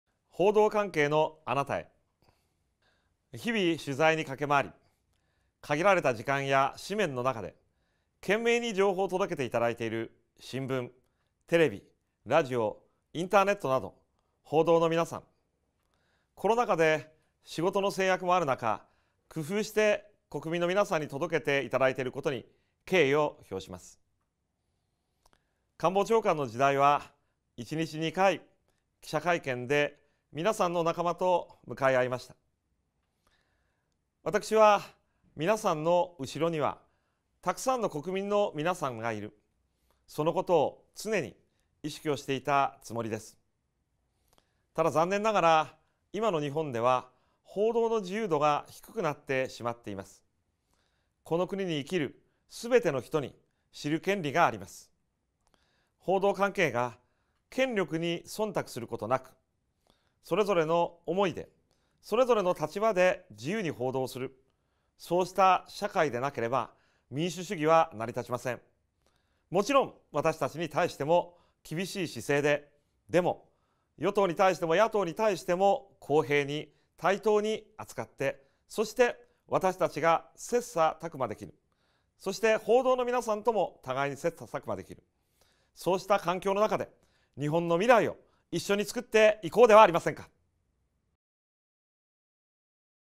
27 報道関係のあなたへ
【報道関係のあなたへ】

日々取材に駆け回り、限られた時間や紙面の中で懸命に情報を届けていただいている新聞 テレビ ラジオ インターネットなど報道のみなさん。コロナ禍で仕事の制約もある中、工夫して国民のみなさんに届けていただいていることに敬意を表します。官房長官の時代は1日2回記者会見でみなさんの仲間と向かい合いました。

わたくしは、みなさんの後ろにはたくさんの国民のみなさんがいる、そのことを常に意識をしていたつもりです。ただ残念ながら、いまの日本では報道の自由度が低くなってしまっています。この国に生きるすべての人に知る権利があります。報道関係が権力に忖度することなく、それぞれの思いでそれぞれの立場で自由に報道をする、そうした社会でなければ民主主義は成り立ちません。もちろんわたしたちに対しても厳しい姿勢で、でも与党に対しても野党に対しても公平に対等に扱って、そしてわたしたちが切磋琢磨できる。そして報道のみなさんとも互いに切磋琢磨できる。そうした環境の中で日本の未来を一緒につくっていこうではありませんか。